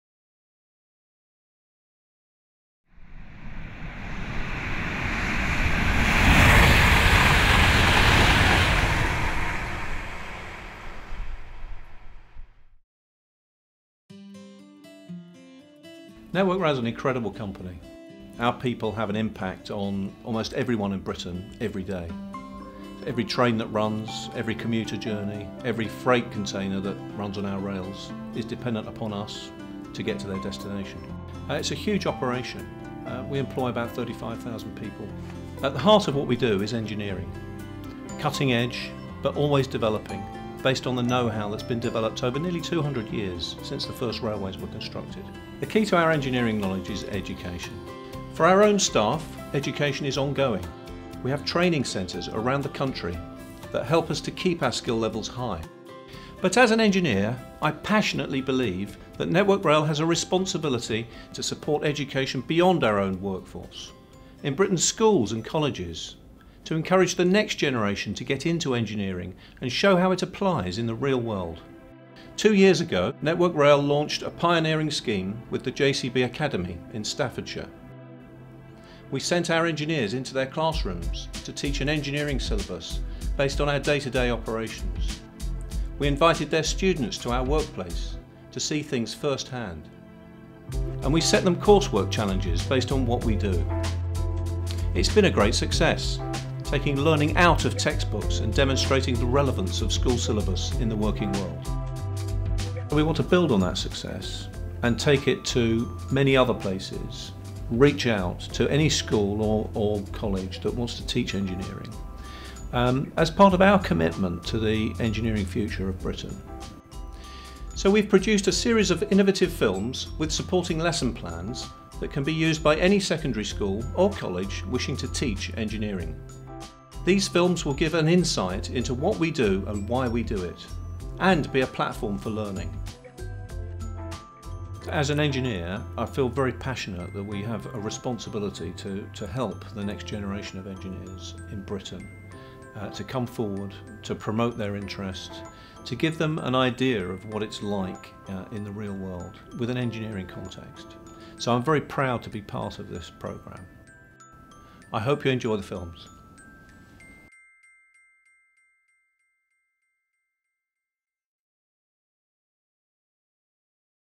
Introduction - Network Rail engineering education (1 of 15)
Steve Yianni, Director of Engineering, introduces this series of films and describes our commitment to supporting engineering education -- within our own workforce and beyond.

For all our national news, campaigns and to see how we're at the heart of revitalising Britain’s railway, please follow: